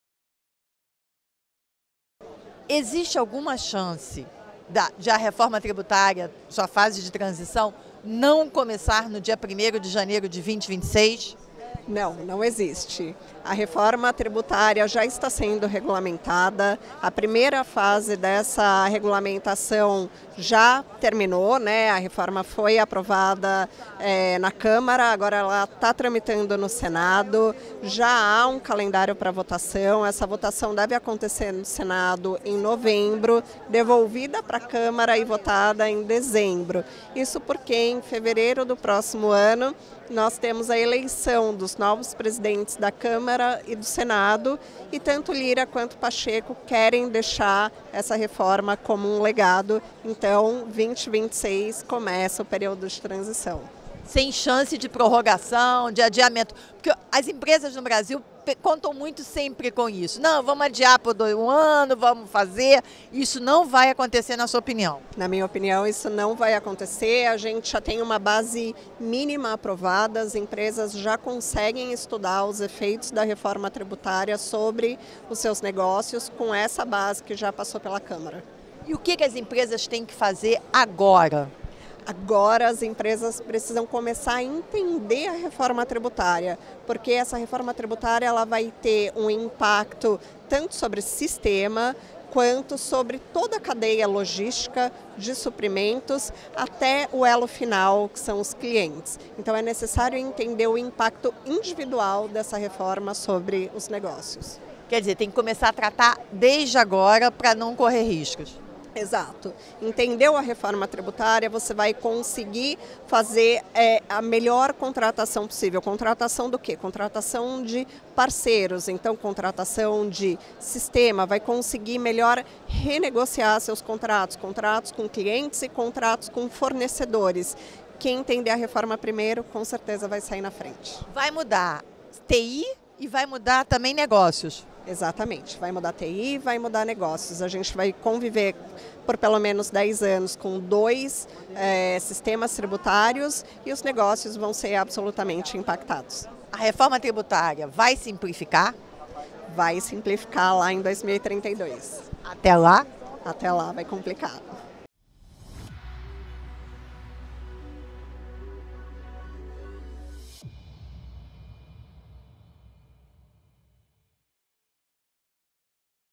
Reforma Tributária: muda a TI, muda negócio e exige atenção imediata
"A reforma vai simplificar a partir de 2032, mas até, lá é bom as empresas entenderem que vai complicar. Haverá a convivência de dois sistemas tributários por 10 anos", afirma a advogada da Bueno Tax Lawyers, Fernanda Lains.